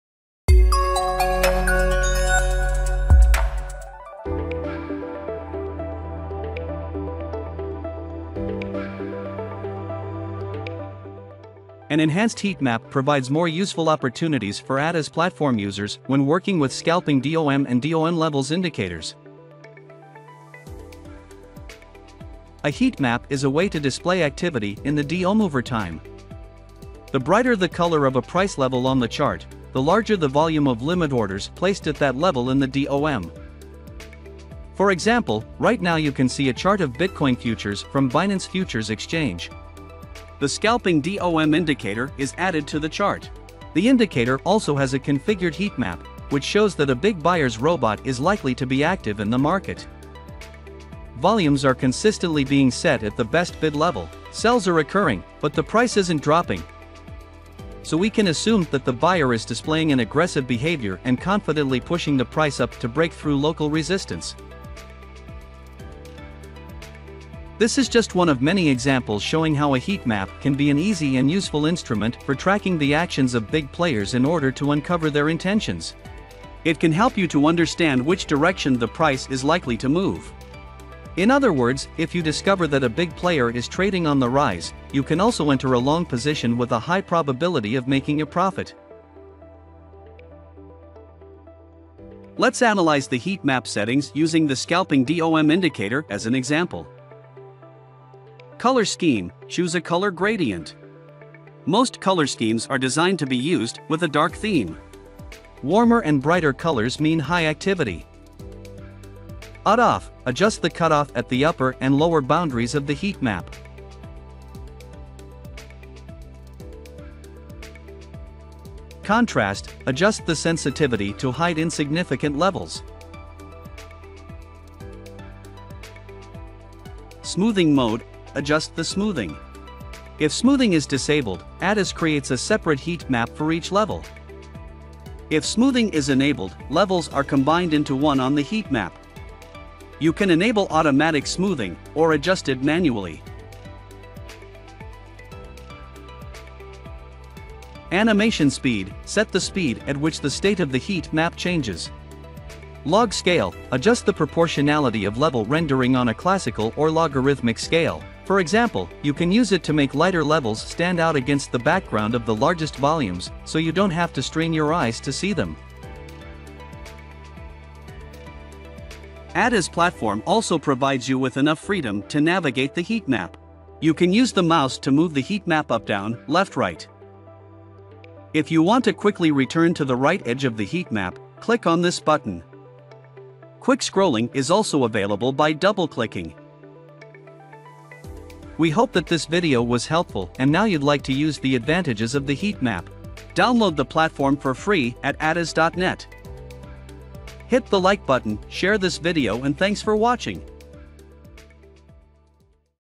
New ATAS functionality – Heatmap!
A hot new feature in the ATAS platform - Heatmap!

ATAS Heatmap features:
○ it is built in Scalping DOM and DOM Levels indicators;
○ it allows you to disclose the intentions of a big player;
○ it allows you to scalp and look for precise entry/exit points;
○ it is available in the Market Replay mode;
○ it is smooth and it has flexible color settings.

Useful links:
👉 Scalping DOM